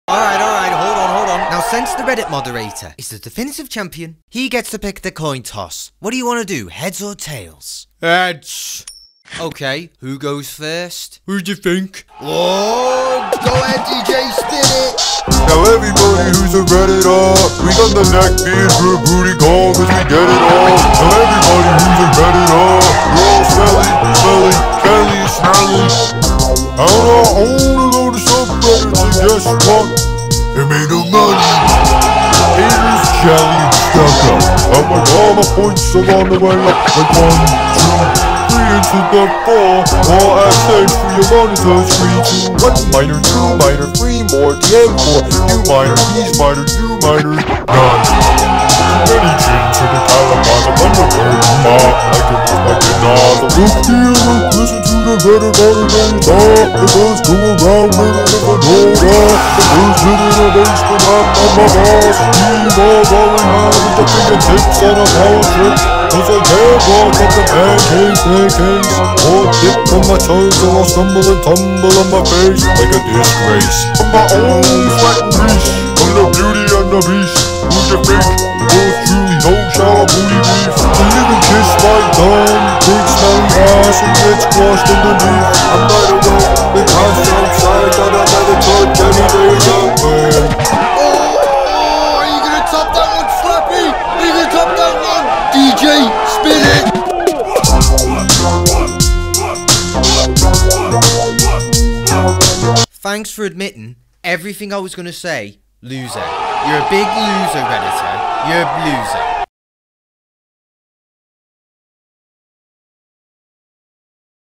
If a Reddit Mod was in a rap battle
In this rap battle video imagine you're battling a Reddit mod in a rap battle, what would you do in this situation.